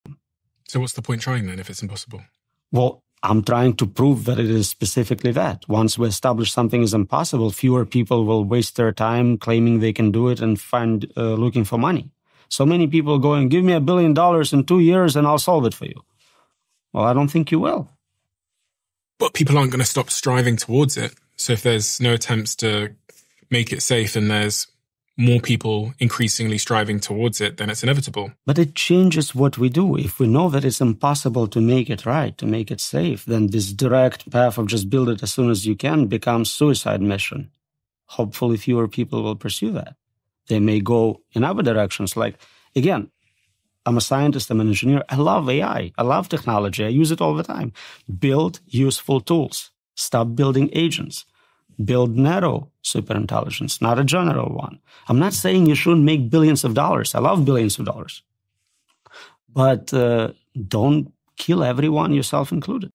AI Safety Why It's Impossible & What To Do
Proving AI safety is impossible might actually make it safer. If the dangers are known, fewer people will blindly chase unsafe solutions. Building useful AI tools, not dangerous agents, could be the better path.